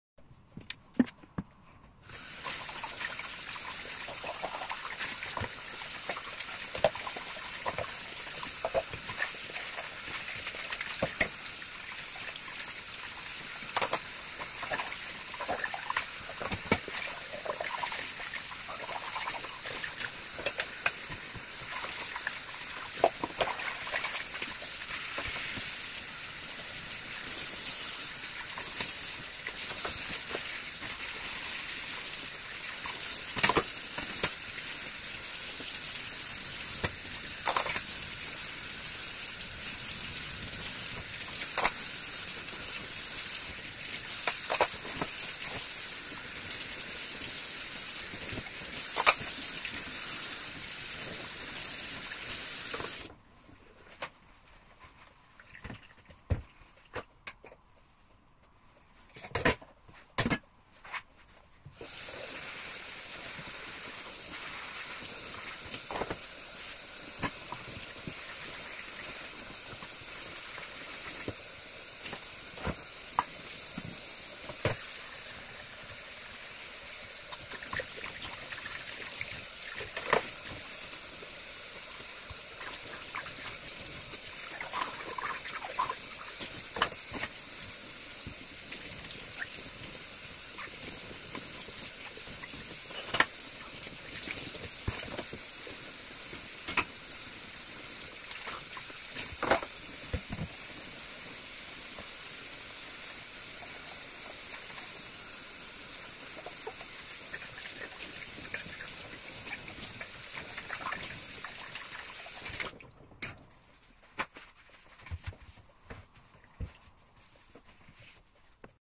2 July 2024 - Relaxation, dishwashing asmr clean with me, cleaning tips #1039
Everyday dish washing.

Cleaning tips.

Washing dishes by hand is a pleasure.

How to wash dishes without heating it in every moment.

How to properly wash dishes.